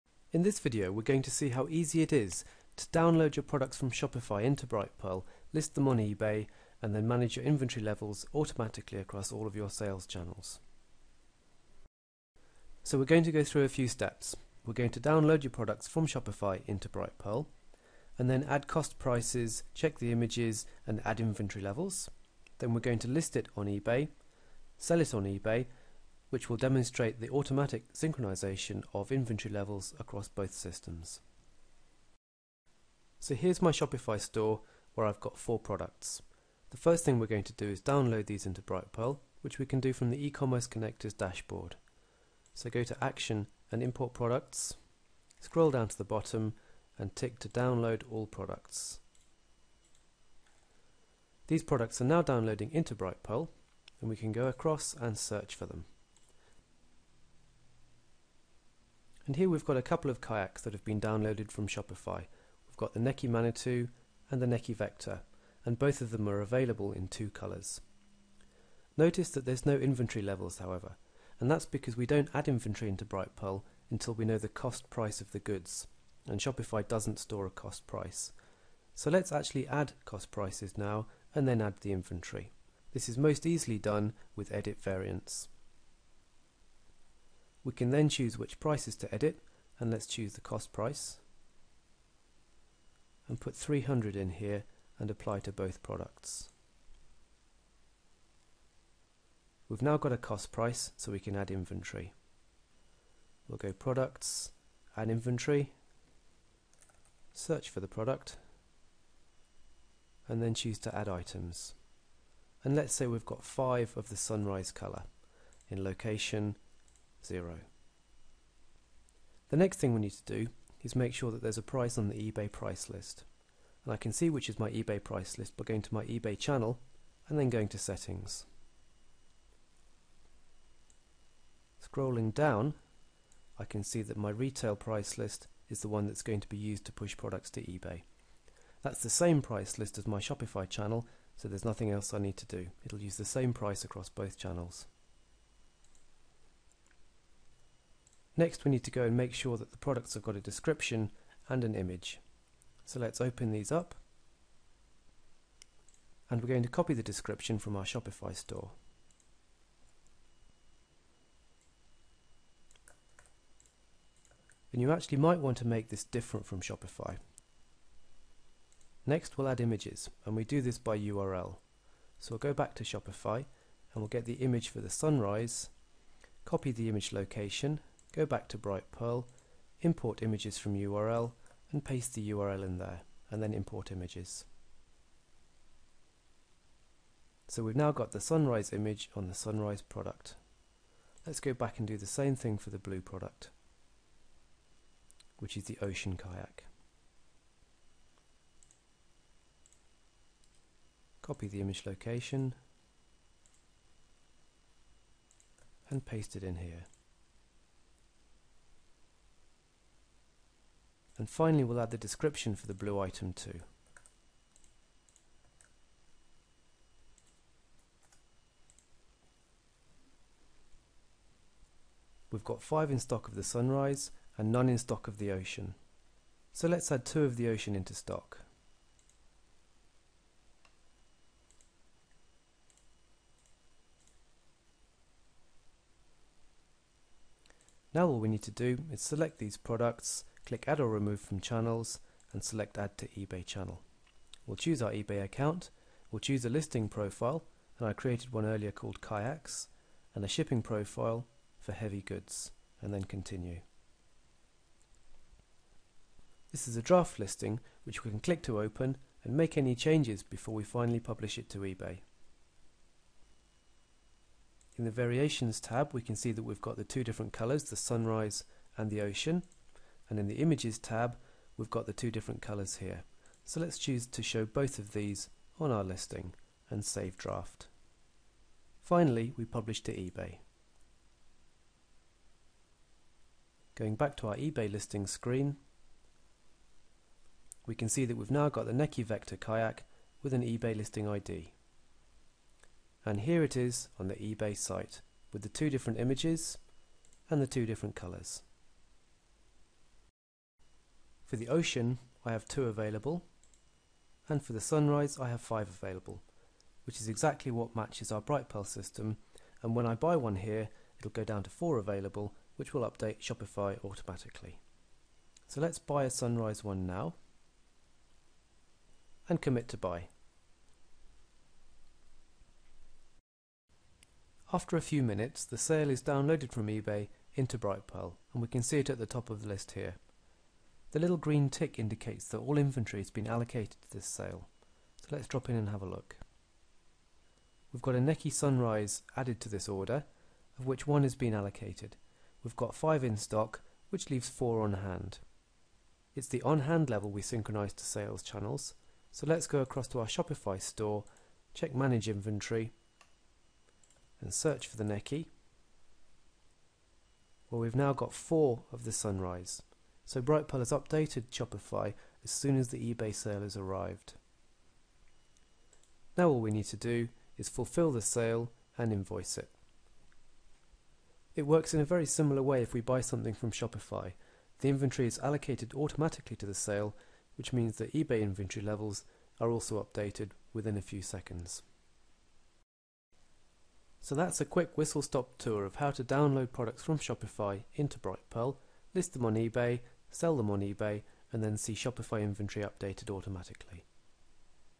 Export Shopify Products to list on eBay | Brightpearl
A video that shows you how to export Shopify products into Brightpearl, add extra information such as cost price, and then list on eBay. Selling the product on eBay allocates inventory, which updates Shopify within a few minutes, preventing you from selling the same item twice, and allowing automatic inventory synchronization across all your sales channels.